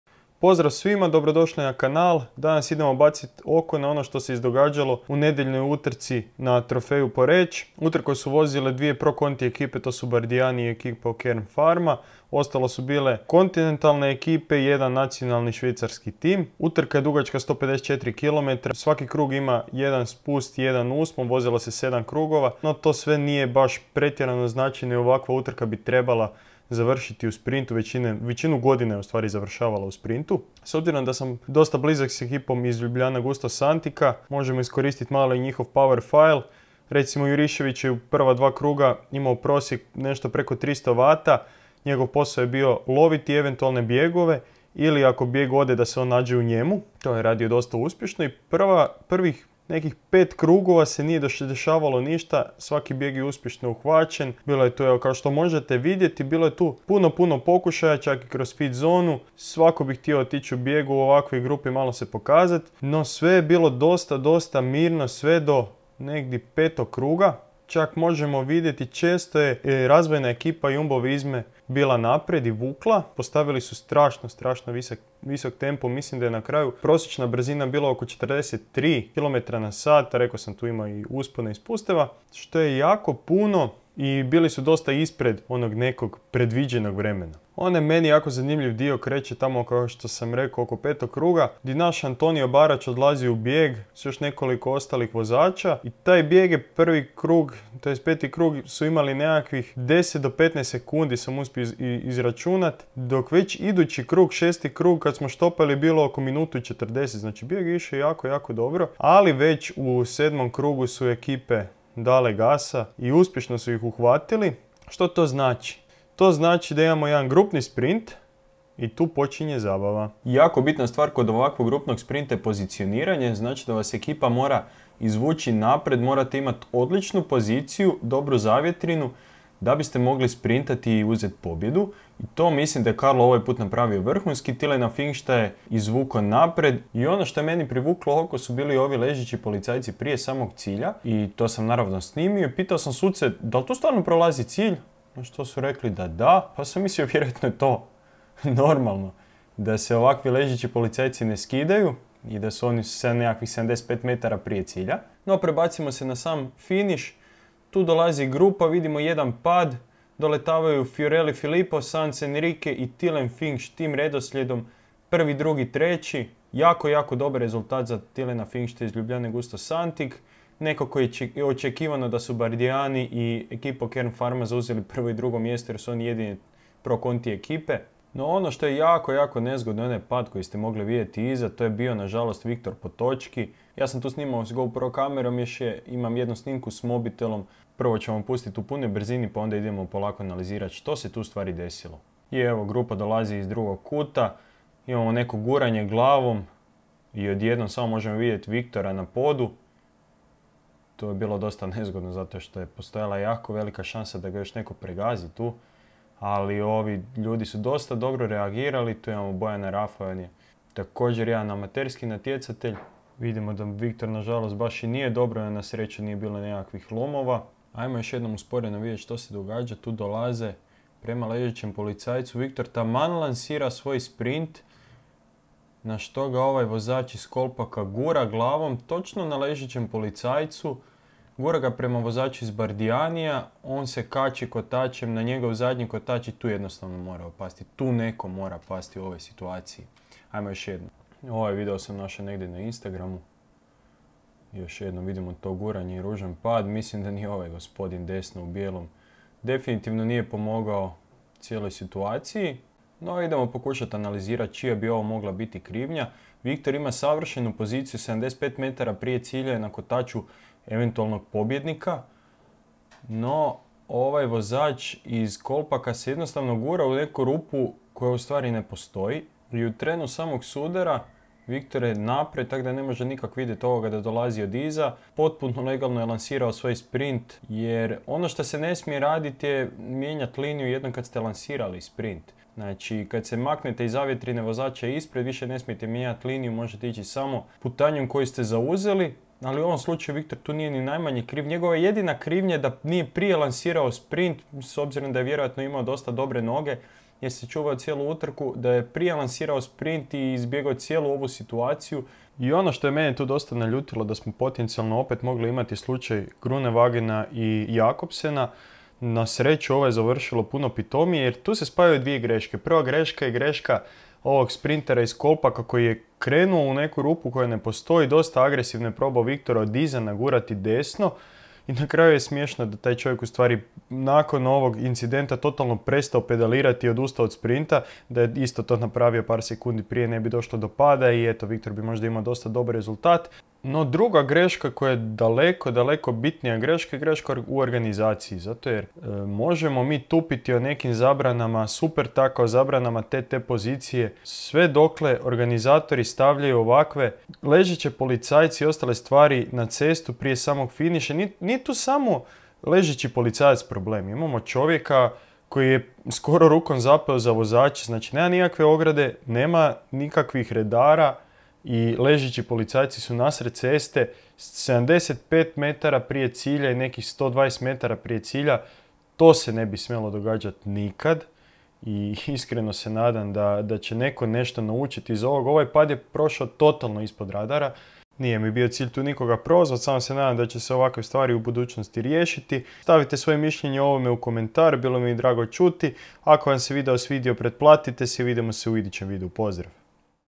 Trofej Poreč | Analiza Utrke
Grupni sprint i pad 75m do cilja na nedjeljnom Trofeju Poreč. Utrku su jake Pro Conti ekipe - Bardiani CSF Faizane i Equipo Kern Pharma i brojne Conti ekipe. To znači vrlo visok prosjek i borbu do samog sprinta.

Viktor je prošao samo s par dubokih ogrebotina i bez lomova.

Čija je ovo greška? Slobodno stavite svoje mišljenje u komentar. Nadam se da će se iz ovog izvući pouka i sigurnost vozača podići znatno višu razinu.